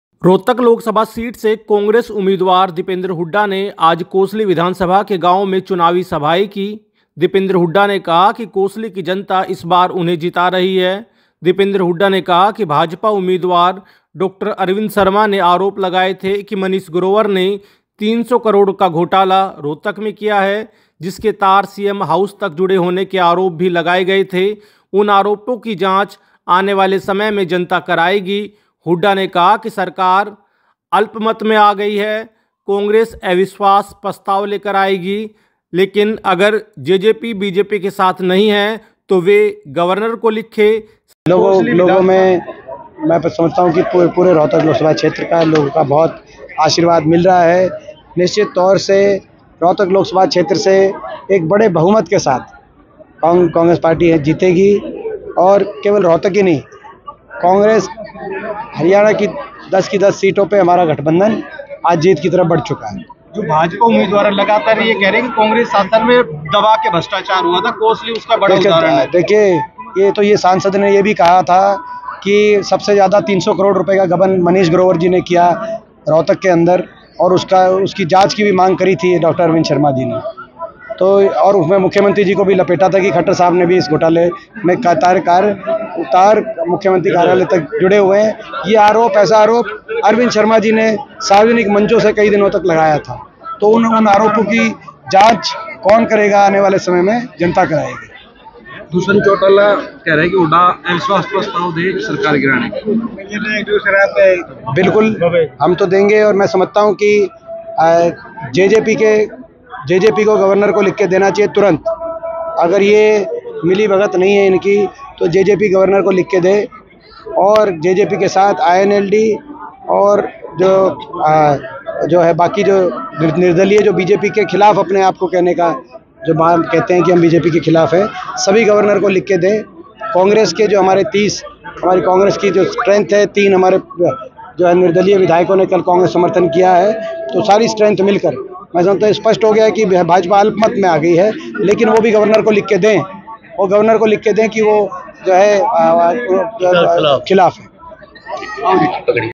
Lok sabha Election | Rohtak Seat | Kosli में दीपेन्द्र हुड्डा की चुनावी सभा | Kosli News Update
रोहतक लोकसभा सीट से कांग्रेस उम्मीदवार दीपेंद्र हुड्डा ने आज कोसली विधानसभा के गांवों में चुनावी सभाएं की.
दीपेन्द्र हुड्डा ने कहा वे बड़े बहुमत के अंदर से इस बार जीत रहें है.
दीपेन्द्र हुड्डा ने कहा कि भाजपा उम्मीदवार डॉ अरविंद शर्मा ने आरोप लगाए थे कि मनीष ग्रोवर ने 300 करोड़ का घोटाला किया था,  जिसके तार सीएम ऑफिस से जुड़े होने के आरोप थे.  उन आरोपों की जाँच आने वाले समय में जनता करायेगी.

हुड्डा ने कहा कि सरकार अल्पमत में आ गई है. कांग्रेस अविश्वास प्रस्ताव लेकर आएगी, लेकिन अगर जेजेपी बीजेपी के साथ नहीं है तो वे गवर्नर को लिख कर दे .